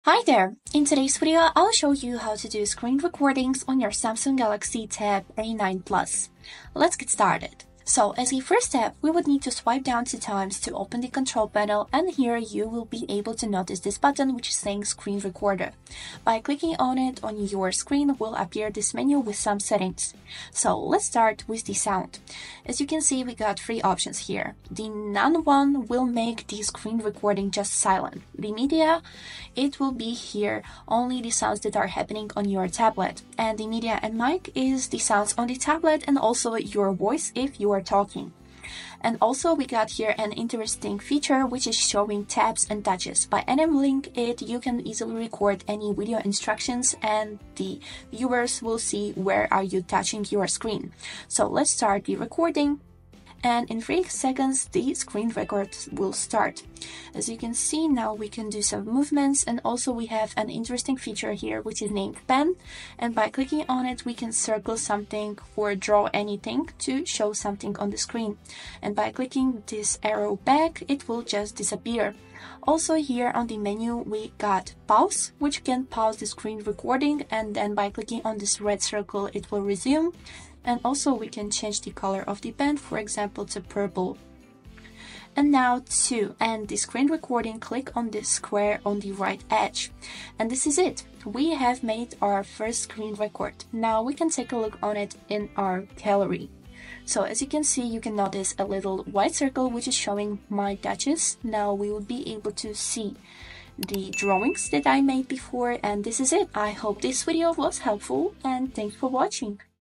How to Record Screen on Samsung Tab A9+
Learn how to record your screen on the Samsung Tab A9+ with this step-by-step guide! Whether you're creating tutorials, capturing gameplay, or saving important moments, this guide covers everything. Discover where to find the screen recording option, how to adjust settings, and how to stop or edit your recordings. Take control of your Tab A9+ and start sharing your screen effortlessly!